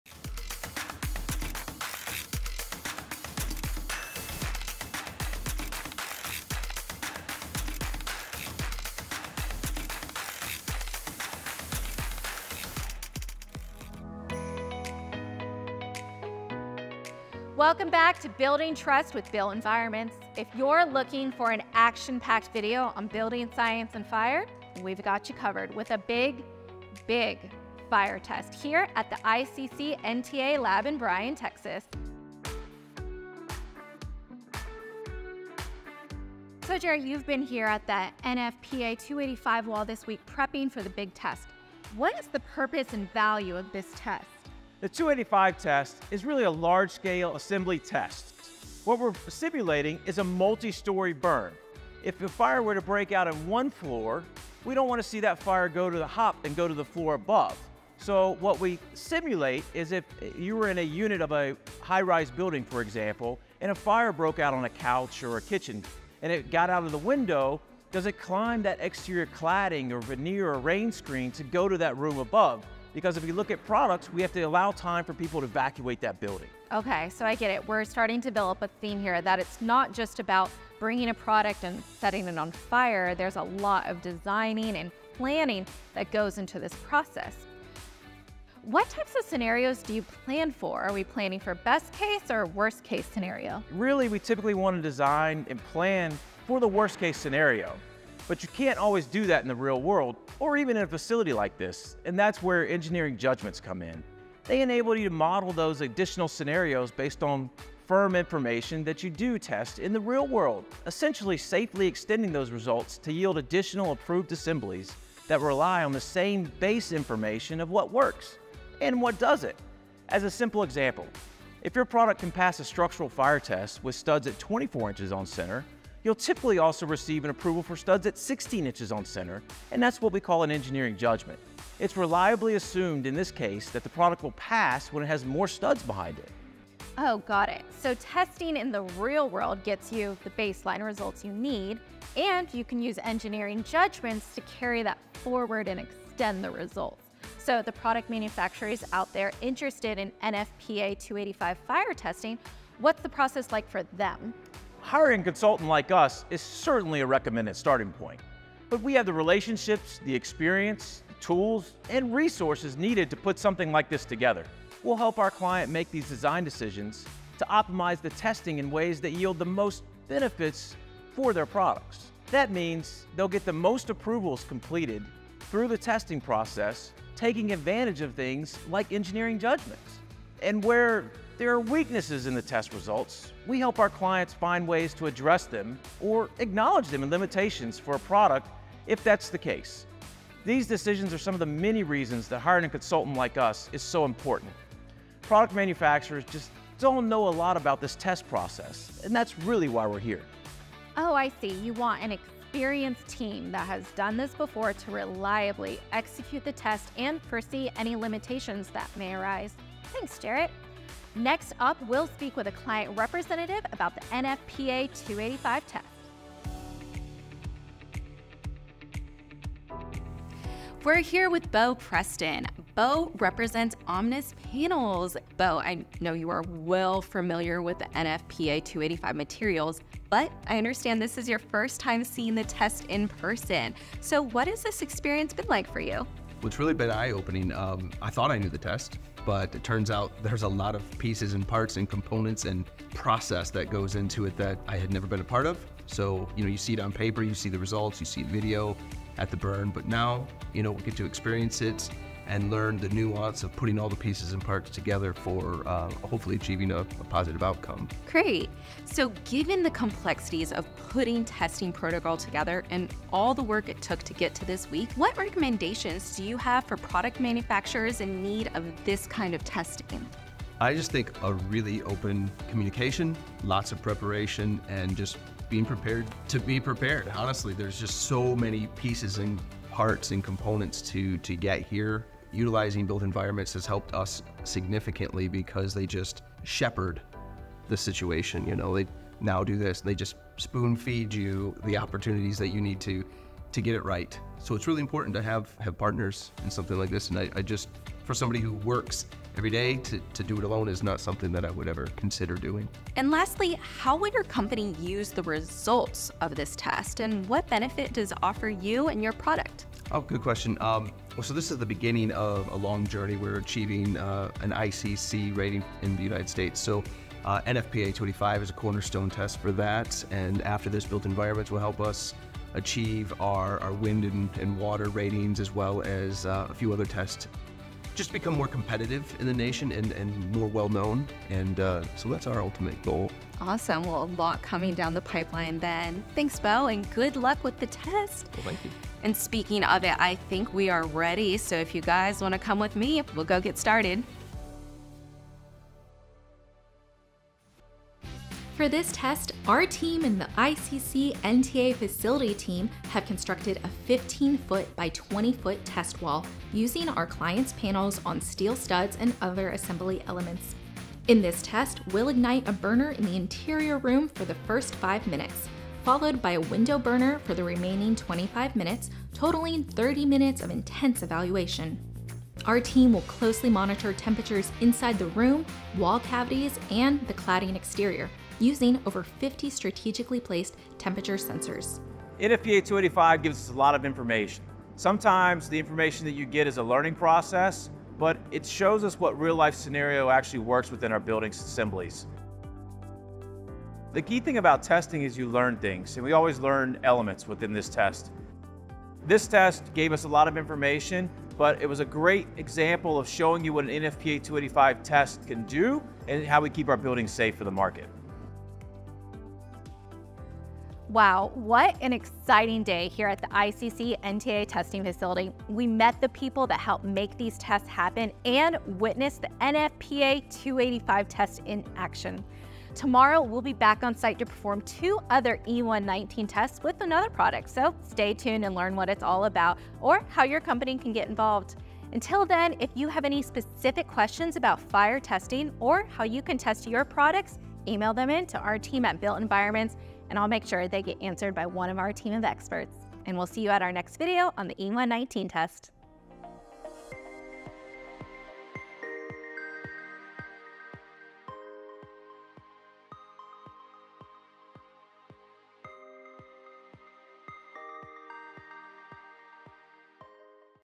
FIRE TEST IN ACTION! Inside look into the NFPA 285 Test at the ICC NTA Testing Facility!
This week we'll show you the NFPA 285 Fire Wall Test in action!  See what it takes to get our clients new product all the way through testing. We were joined by our client Beau Preston of Omnis Panels!

Next Week we will finish by showing you the E119 Test Wall with Innovation US MgO

Send in or comment your questions now to be featured in our Upcoming Discussion Video on fire testing with Jarrett and Steve!